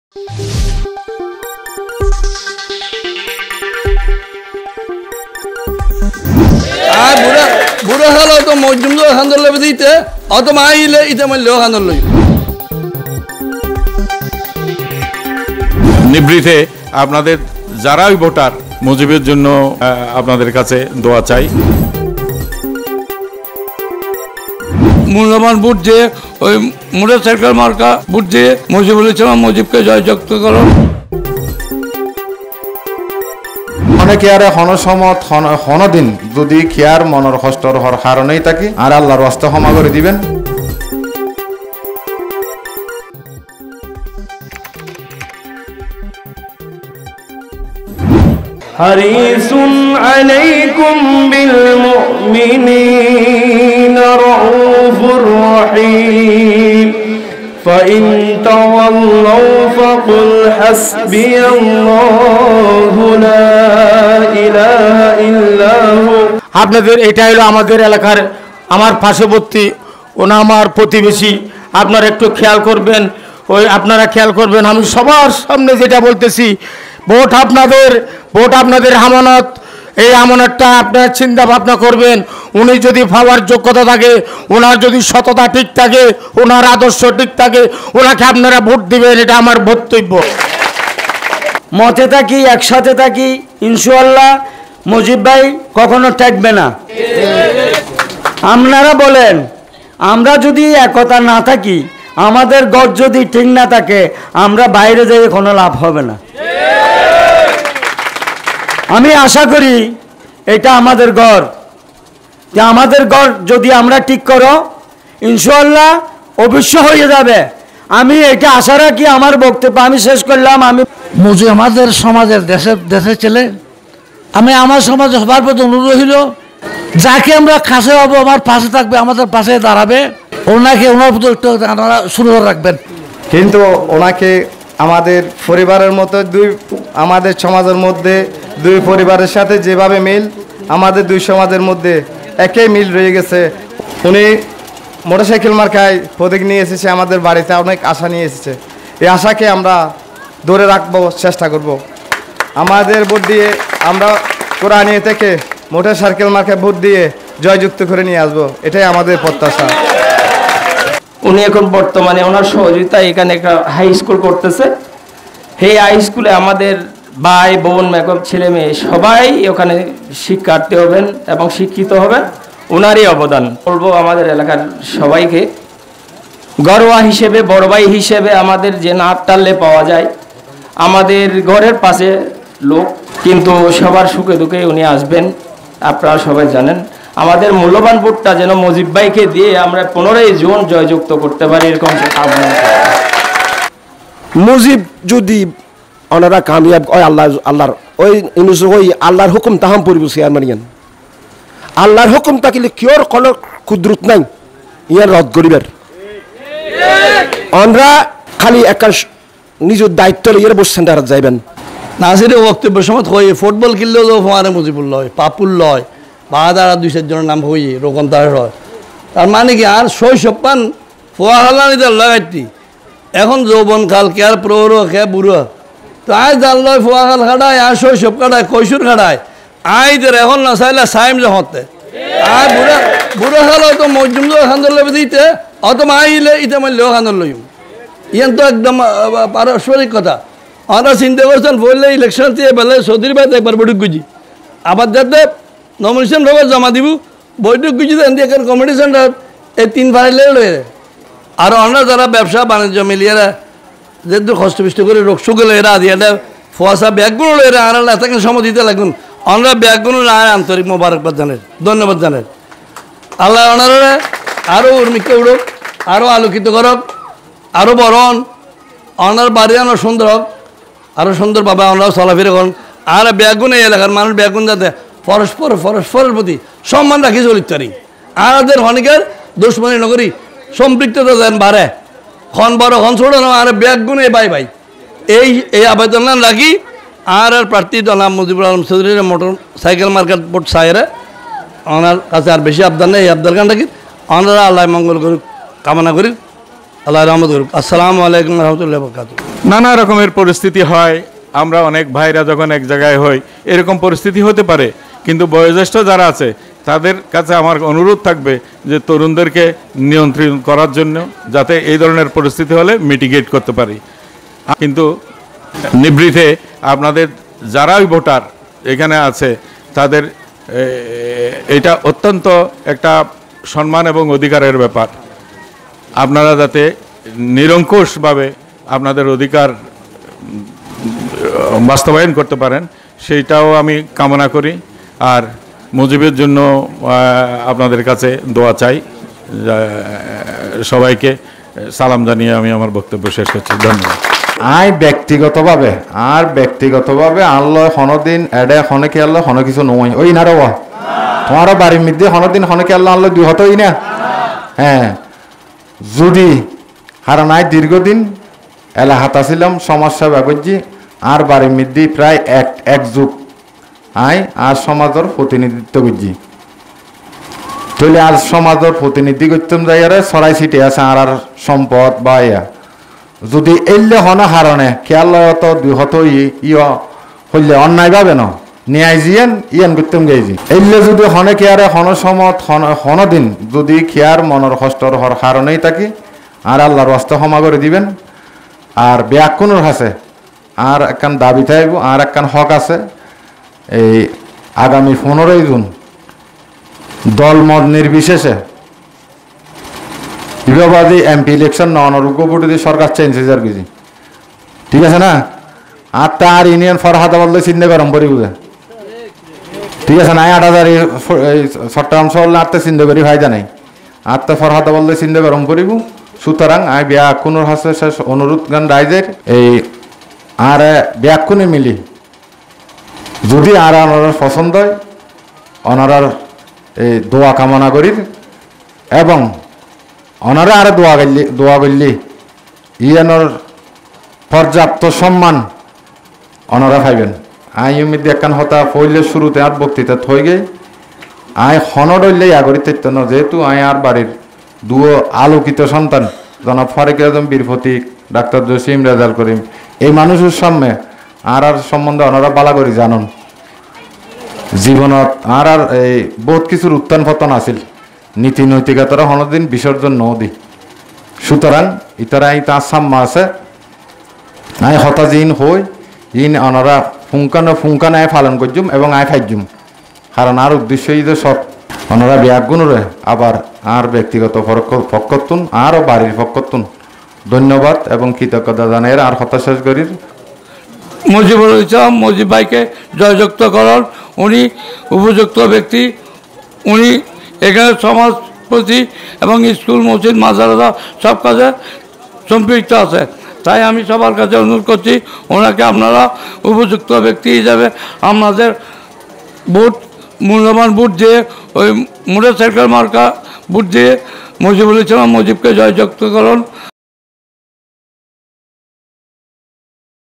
মোঃ মুজিবুল আলম চৌধুরীর মোটর সাইকেল সমর্থনে ৫নং ওয়ার্ডে ছৈয়দ বাড়িতে নির্বাচনী মতবিনিময় সভা
আলাপ টিভি ALAP TV